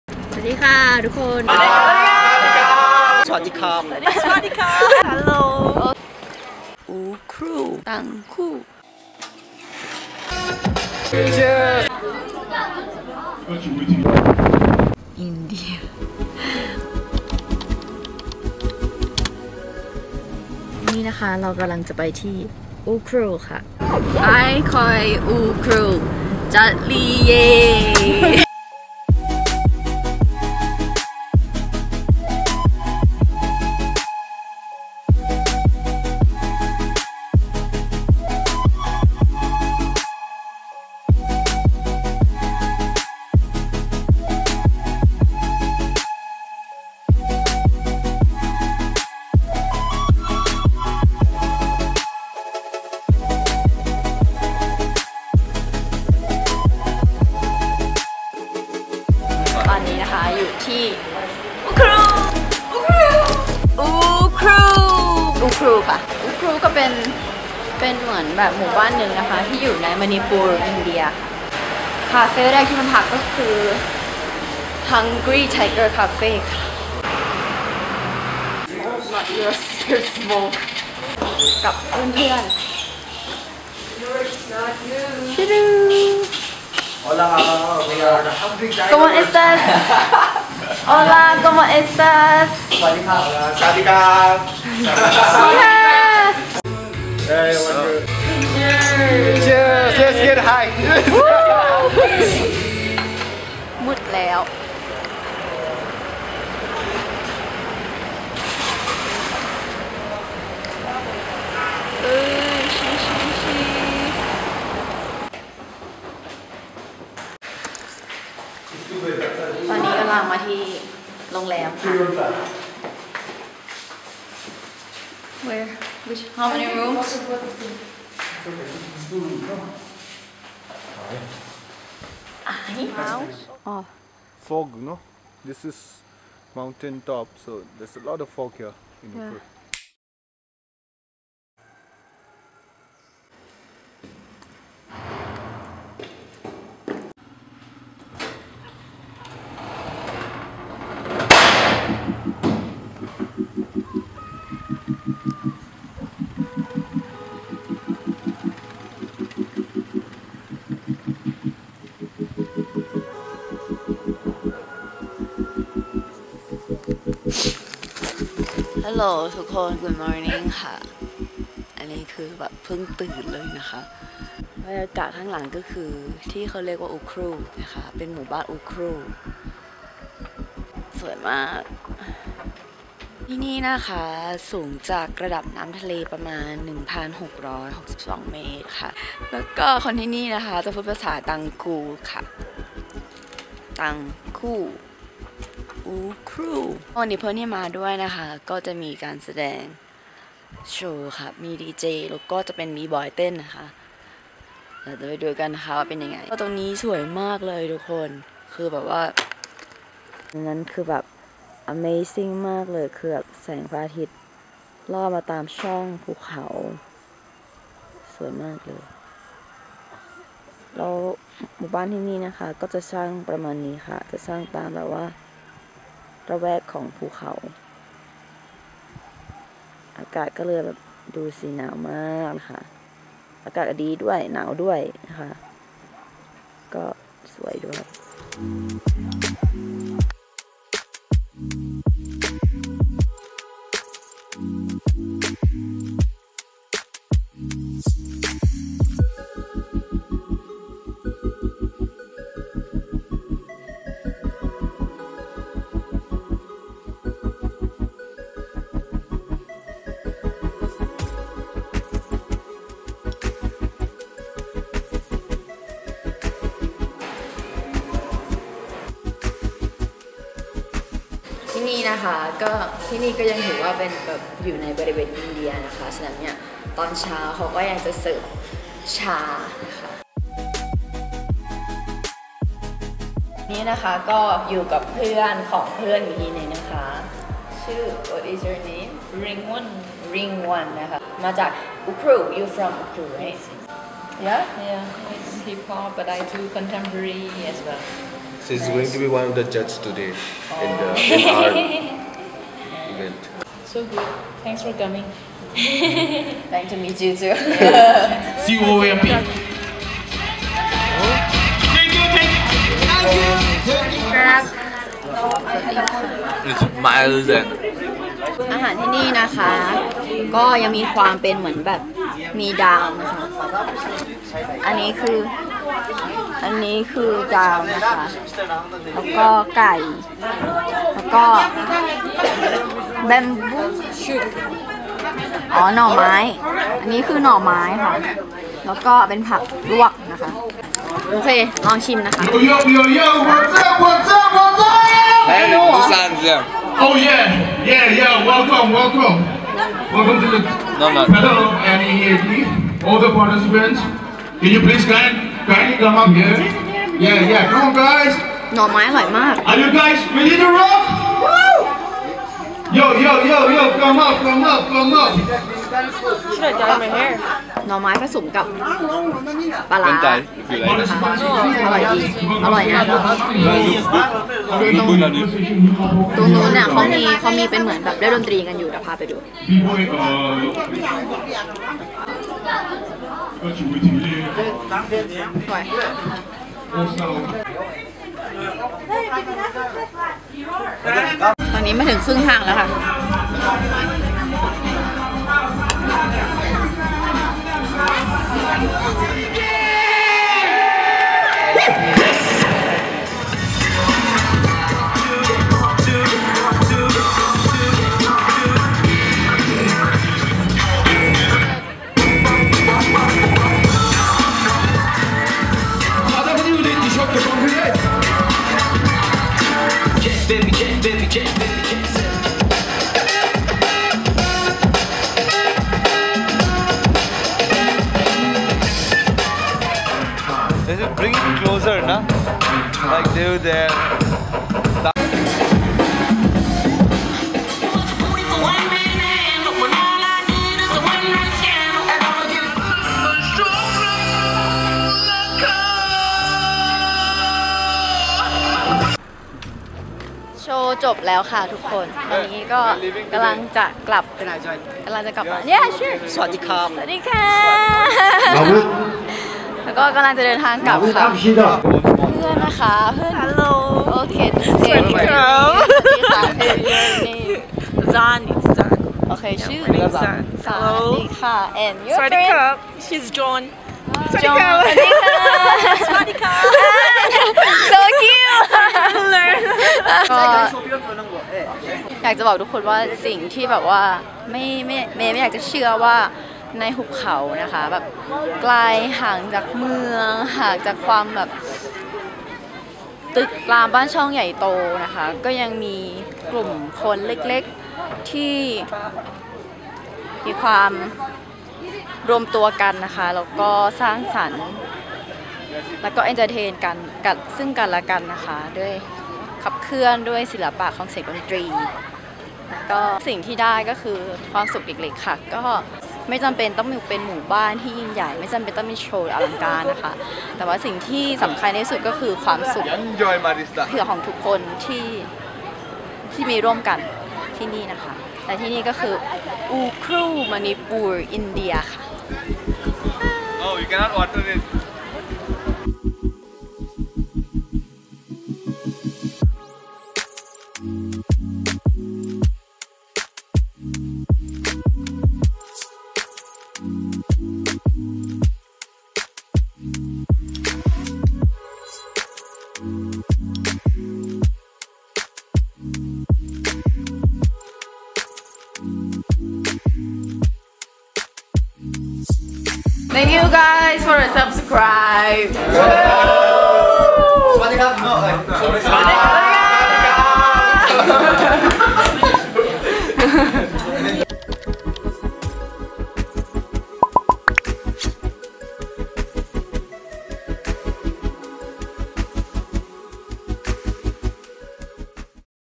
First time journey to Ukhrul from Imphal,Manipur northeast India|เพื่อนอินเดียดนตรีในหุบเขารัฐมณีปูร
Thai female travelled to Northeast India 😇❤️

Facebook page and Instagram: Maynumpetch

*Dal คือ เมนูอาหารอย่างนึงที่มีส่วนประกอบหลักคือถั่ว คนอินเดียมักนำเมนูมาผสมกับข้าว
Some useful “Thai” words
*sawasdee(สวัสดี) means hello
*peun(เพื่อน) means friends
*cheu(ชื่อ) means name

To be honest, I have never imagined that such a place that surrounded by mountains and nature which is so far away from the hectic city and from big buildings. There’d still be some small group of people that gathering together, entertaining to each other and inspiring each other with the art of music. What I really found here is “Happiness”. It doesn’t matter whether we are a big group of people or whether the show is huge or magnificent or not. But the important thing is “Happiness” that we share and laugh together. And here is Ukhrul, Manipur, India”. May said in this video
Three words for this cherish trip from me
“Friends art and journey”.

“เมไม่อยากจะเชื่อจริงๆว่า สถานที่ที่หนึ่งที่ซึ่งห่างไกลจากความเป็นเมือง จากตึกรามบ้านช่องใหญ่โต ก็ยังคงมีคนกลุ่มเล็กๆซึ่งมีการรวมตัวกัน สร้างสรรค์สร้างความบันเทิง และเป็นแรงผลักดันให้กันและกันได้สร้างผลงานและขับเคลื่อนไปพร้อมกันด้วยศิลปะของเสียงดนตรี สิ่งที่เมสัมผัสได้ที่นี่คือ”ความสุข” ไม่จำเป็นต้องเป็นชุมชนที่ยิ่งใหญ่ ไม่จำเป็นต้องมีโชว์การแสดงที่อลังการหรือใหญ่โตโอ่อ่า แต่สิ่งที่สำคัญที่สุดคือ”ความสุข” ที่ผู้คนที่นี่มีร่วมกัน แบ่งปัน หัวเราะและใช้เวลาร่วมกัน และที่นี่คืออูครูล มณีปูร อินเดีย” คำพูดที่เมกล่าวไว้ในวิดีโอ

Thank you for a wonderful music
Musician: Jef